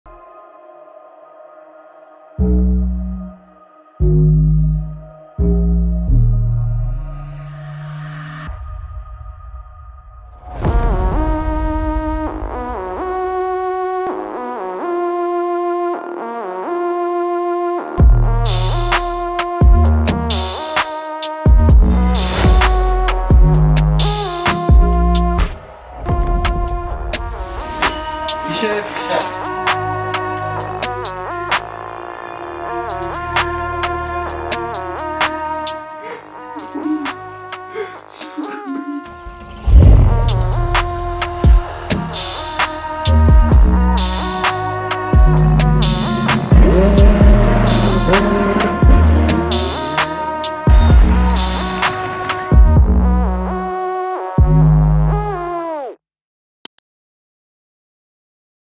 Showreel 2023 Images Koncept
Voici le showreel 2023 d'Images Koncept, vous y retrouverez les meilleurs projets de l'année condensé dans cette seule vidéo. Allant d'événements sportifs, passant par des publicités jusqu'à des documentaires.